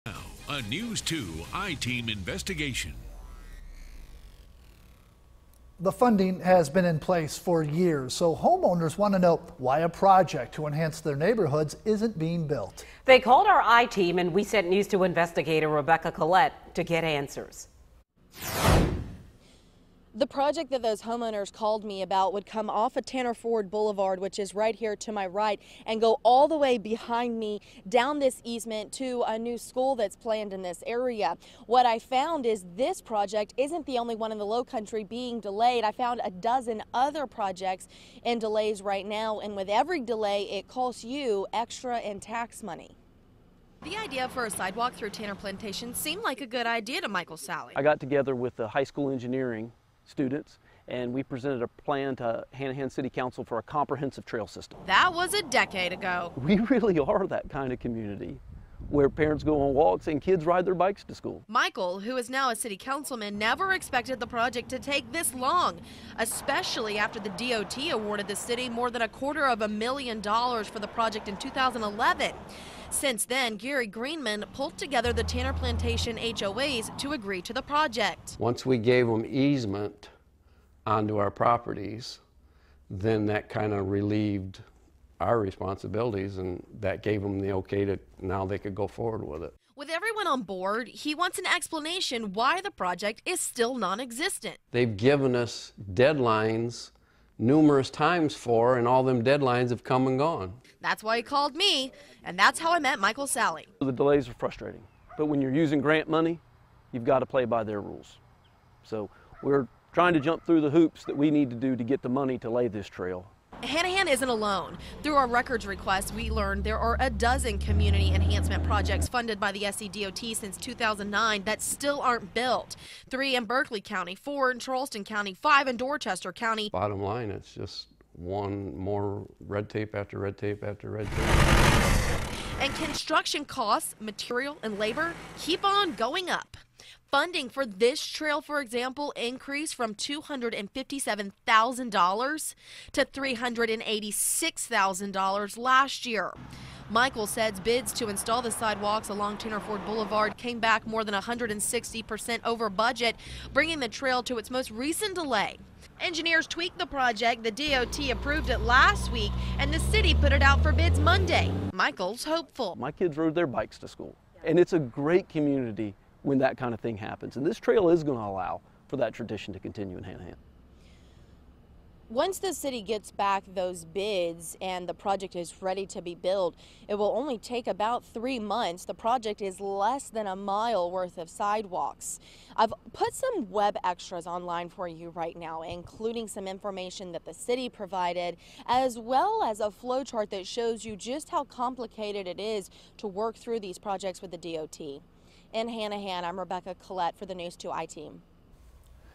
News 2 I-Team: Lowcountry projects funded, incomplete
When the News 2 I-Team started digging, we found several sidewalk projects across the Lowcountry have been delayed for years even though money is in place to build them.  With every delay, the projects get more expensive.